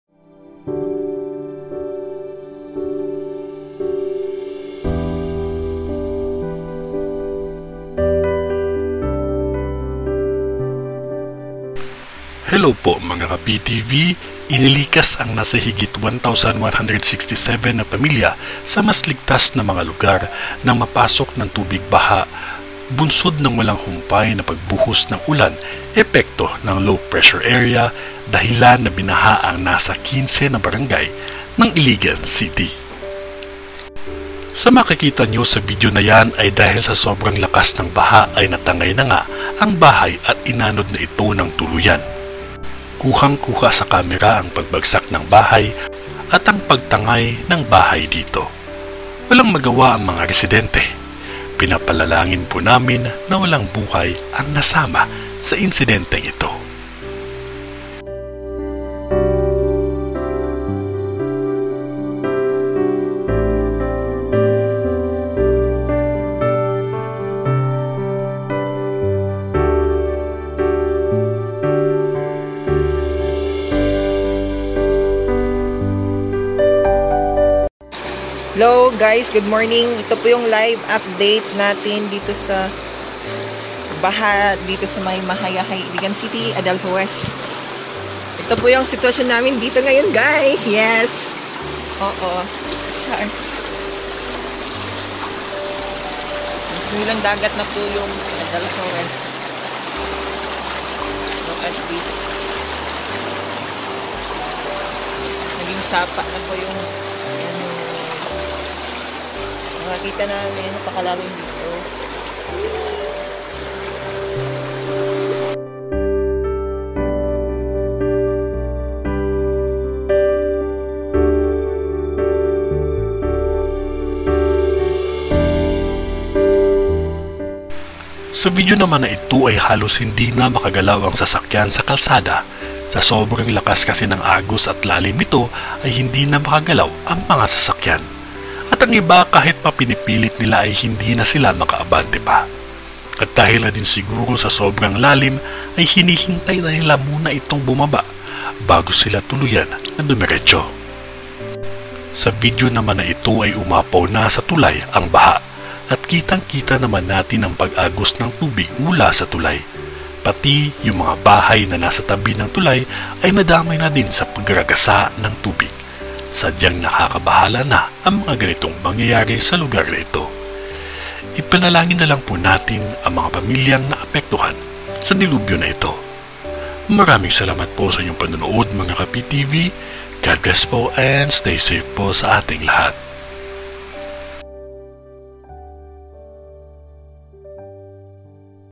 SHOKCING! ANG MATINDING PAGBAHA AT PAGBAGSAK NG BAHAY SA ISANG LUGAR SAPUL NA SAPUL SA VIDEO
JUSKOPO! ANG APAT NA TUMPAK, NAGKATOTOONG PREDIKSYON NI NOSTRADAMUS SA PILIPINAS AYUN SA MGA ISKOLAR | PROPESIYA TV